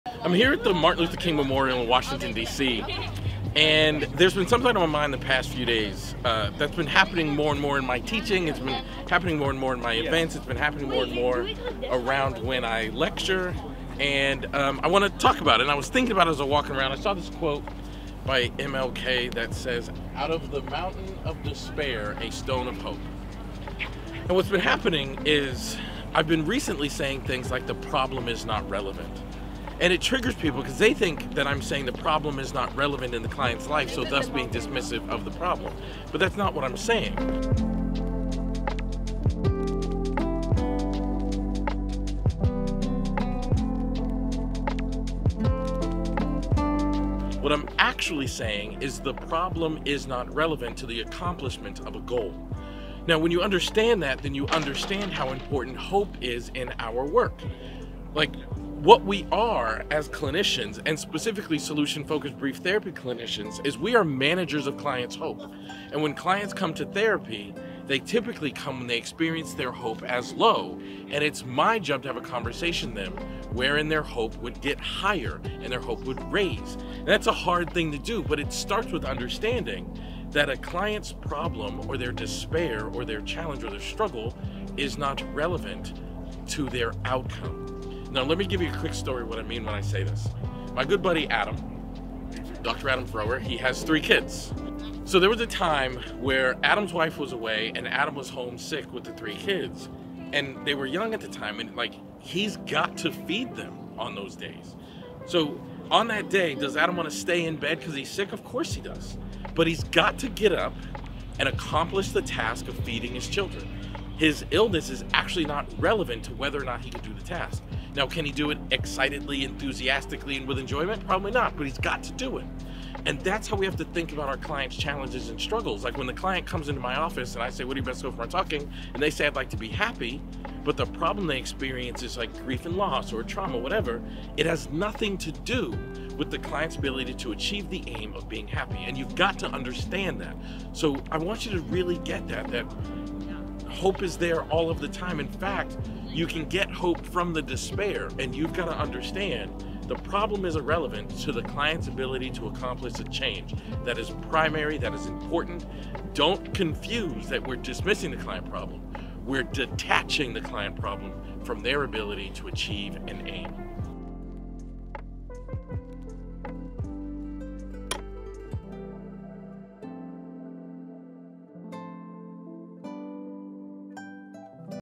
Why the Problem Isn't Important in Your Therapy Session | SFBT Moments 336
One mistake we often make as psychotherapists is focusing way too much on the problem and less about what our clients truly want. Let's discover how we can get to the desired outcome without getting distracted by the details of the problem.